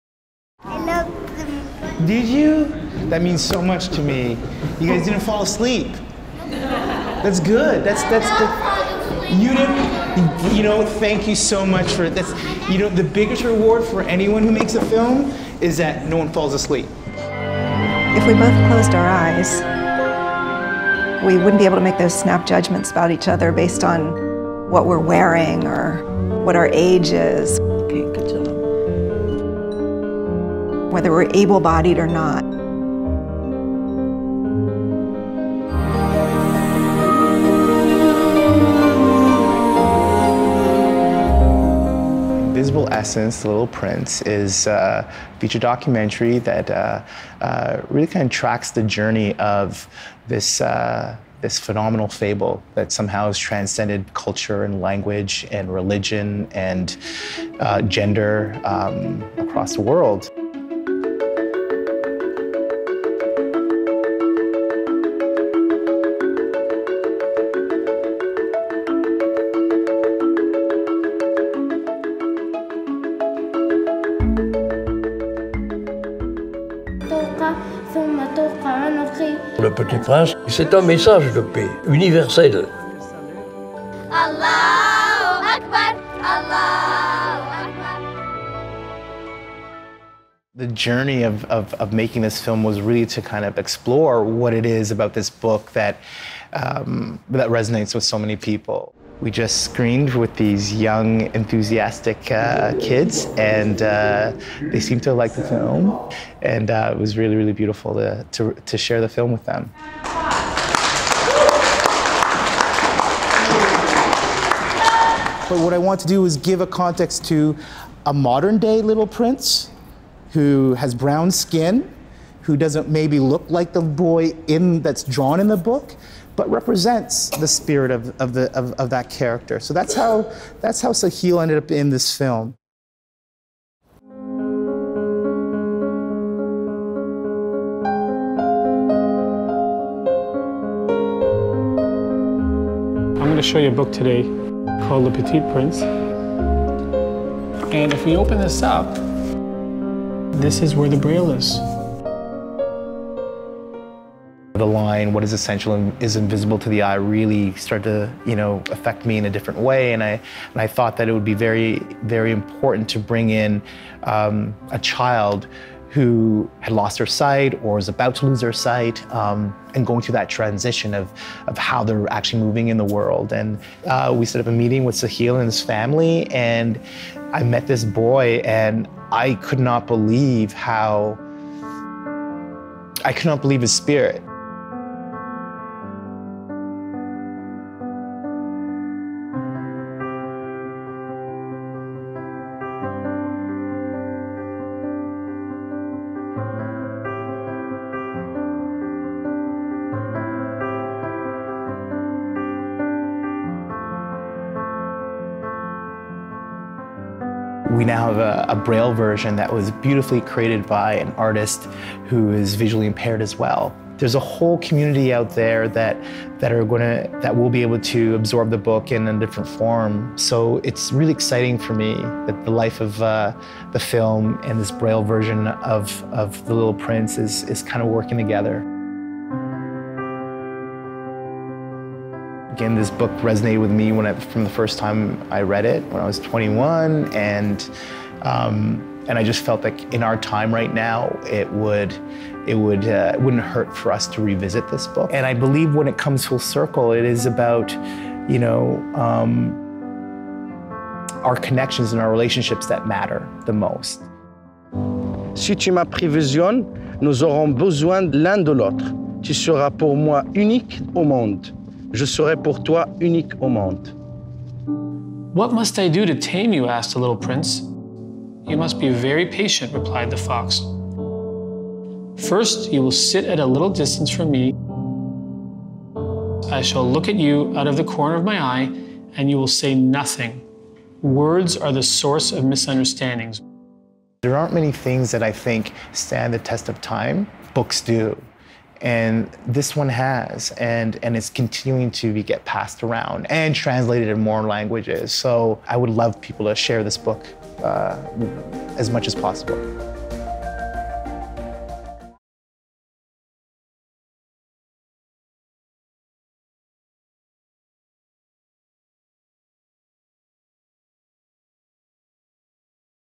What makes The Little Prince so special?
Filmmaker Charles Officer goes looking for what has made The Little Prince a book that's resonated with people for decades.

The Little Prince is a short novel about a displaced boy from another planet who forms close friendships and negotiates sadness — all weighty concepts for tiny readers. It's the kind of book you get something different from reading as a kid than you do as an adult. It's also the most-translated non-religious text ever (just to give you a sense of how much it's meant to a lot of people).

In his new documentary produced in collaboration with the CBC called Invisible Essence: The Little Prince, Charles Officer looks at the way the 1943 book by Antoine de Saint-Exupéry has traversed decades and cultures to remain an evocative tool for learning about your own heart. And he introduces Sahil, a young boy who finds his own way through The Little Prince.

In this video made by filmmakers Istoica, Officer gives you a glimpse into what makes The Little Prince so special — and why it still means so much to him today. As he explains: "This book resonated with me from the first time I read it when I was 21, and I just felt that in our time right now it wouldn't hurt for us to revisit this book. And I believe when it comes full circle, it is about our connections and our relationships that matter the most."